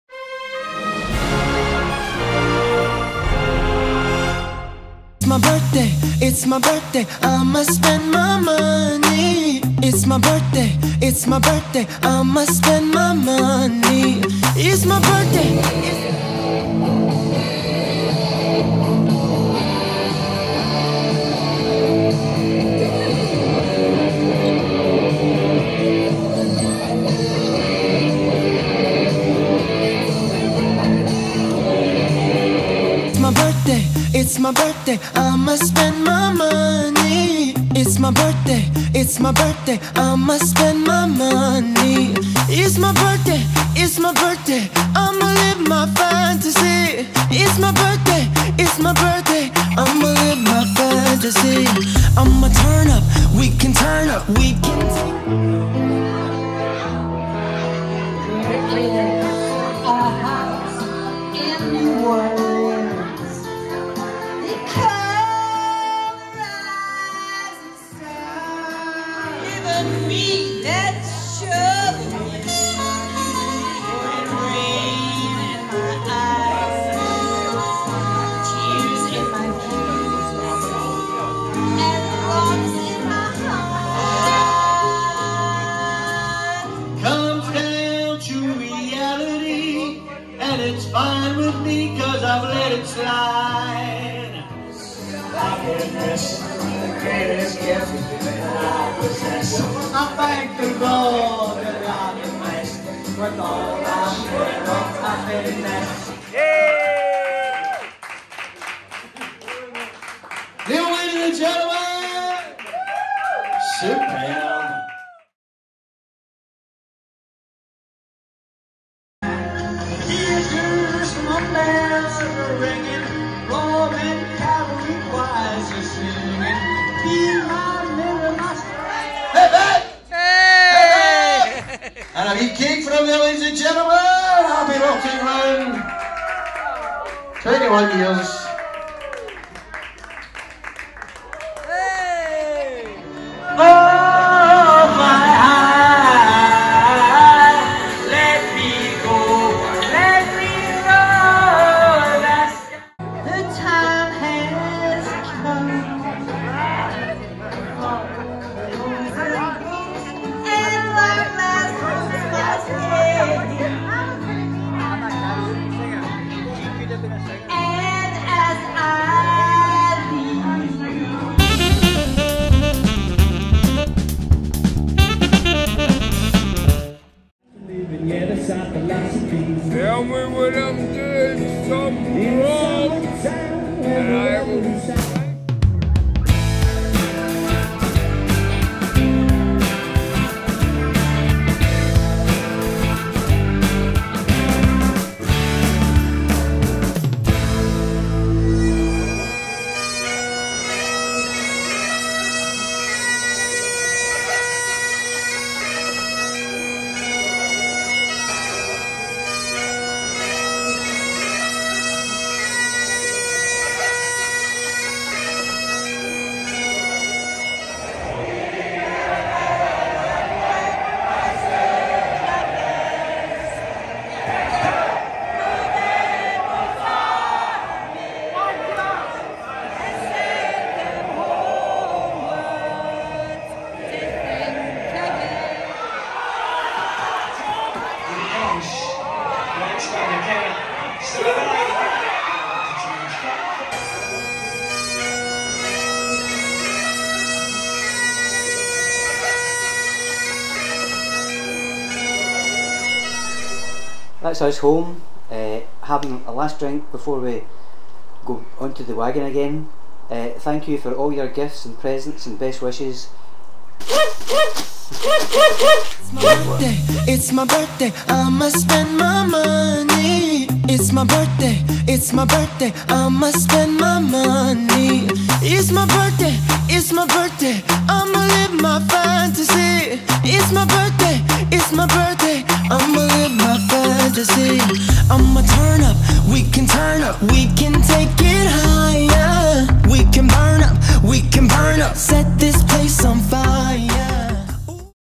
75th BIRTHDAY WEEKEND
This is us celebrating my 75th birthday in Glasgow after a party organised by Irene Hastie had to be cancelled due her sustaining a serious injury.